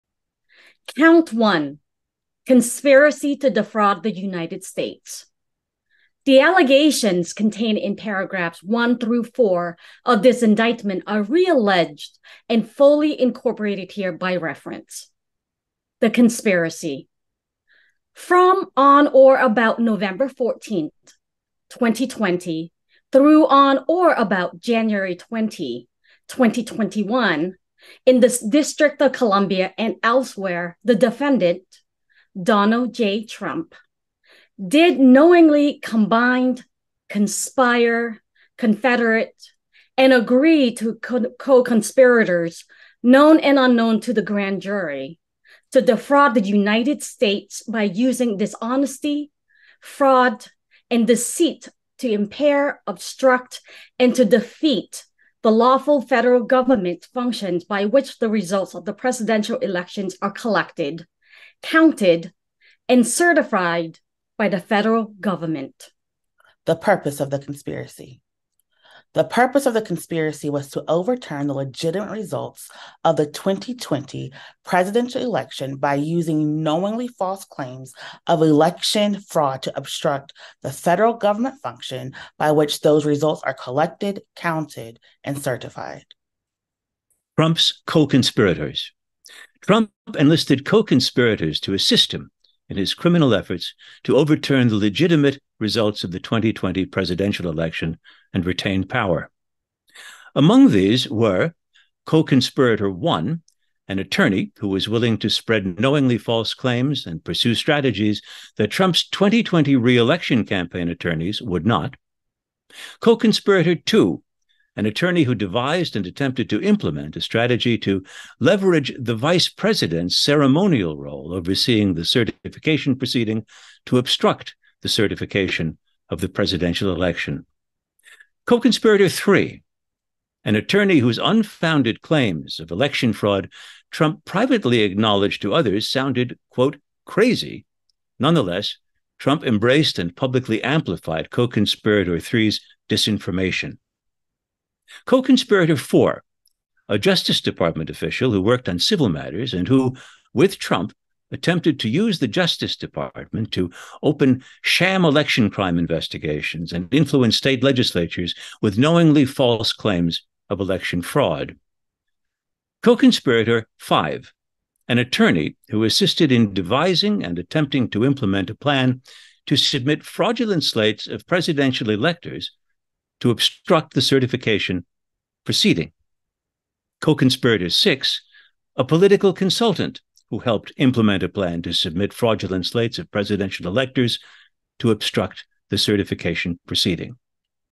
Read the Indictment Episode 2
The federal indictment against Donald J. Trump for election interference.
Pages 3-5